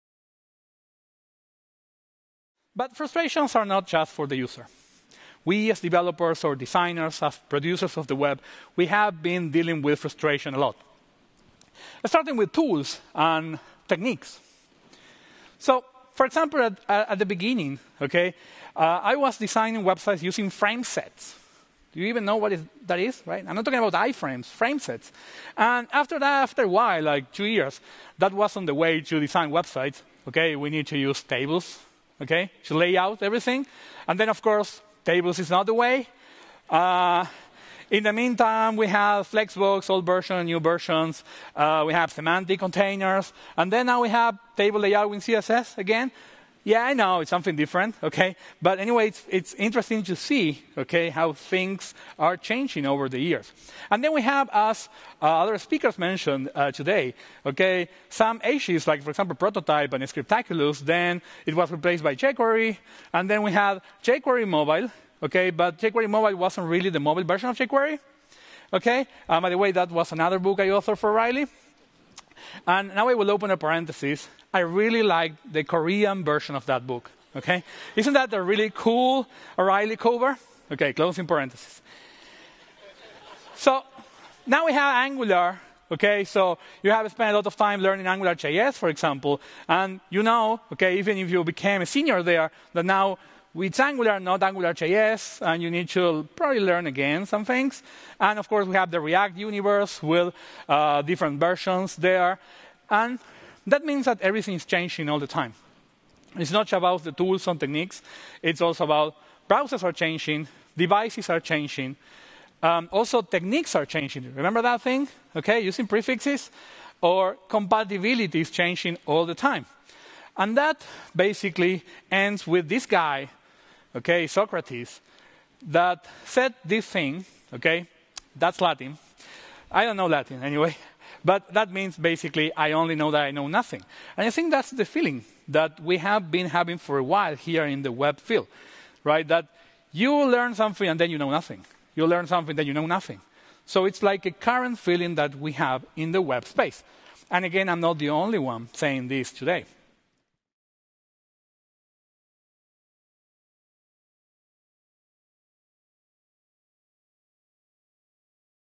Reflecting on 20 years on the web - Maximiliano Firtman (ITMaster Professional Training)
Is it better or easier to work on the web today, 20+ years after its creation? What lessons have we learned—and what are we still doing wrong? Max Firtman explains why, after years of adding layers of complexity to the frontend, especially in relation to layout, compatibility, and performance issues, it makes sense to go back to the roots of the web.